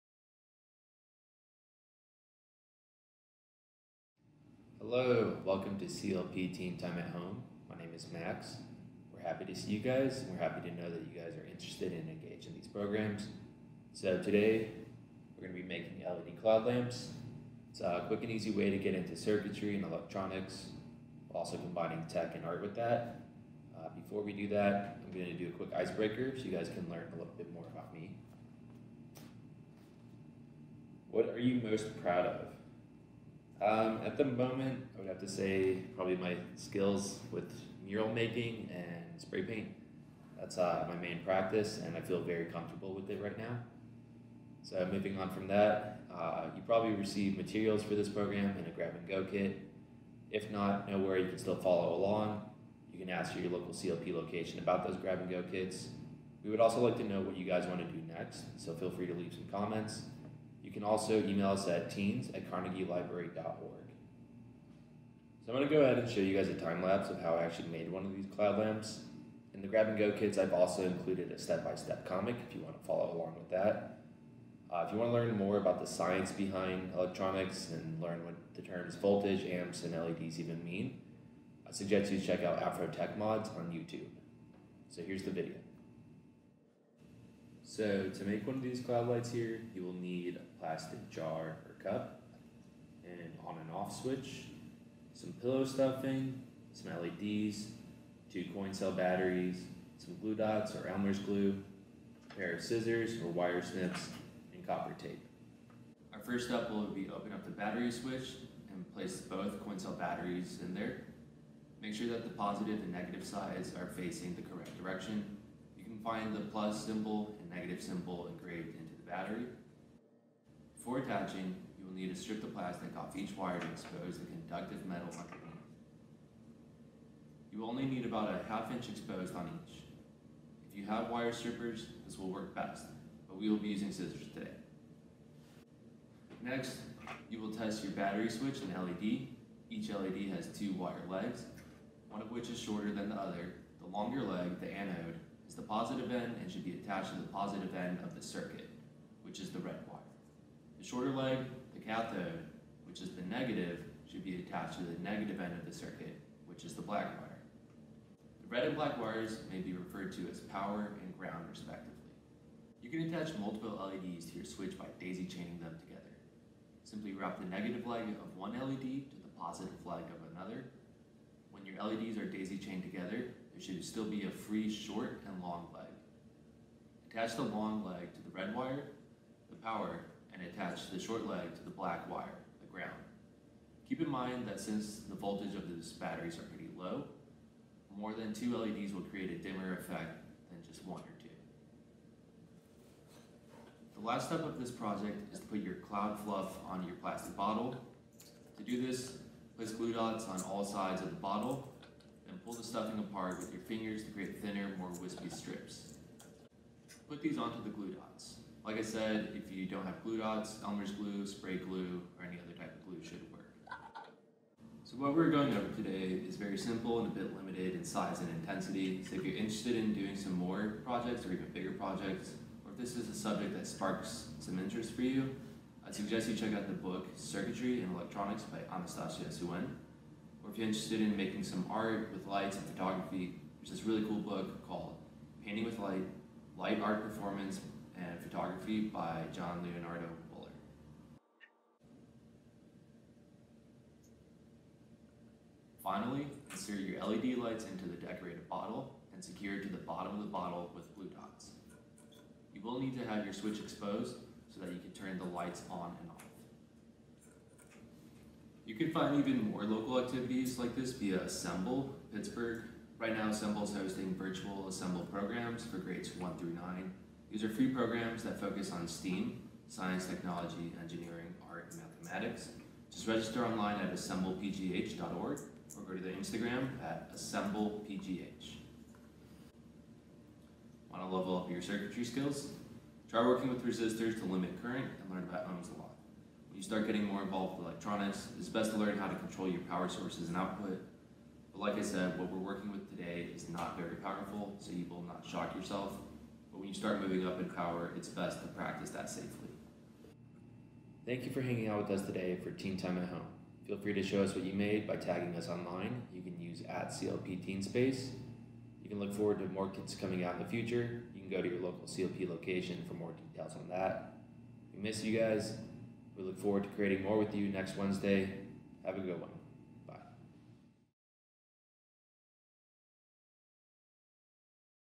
Teen Time @ Home: LED Cloud Lamp
Join us Wednesday afternoons for Teen Time @ Home! Every other week we'll be posting a Teen Time activity that goes along with a Grab & Go Activity Kit available to you for pick-up at CLP locations. Each kit includes all the materials you need for the project.

This week, Max will show us how to make a cloud lamp using LED lights and batteries! Kits for this project are available at all open CLP locations while supplies last.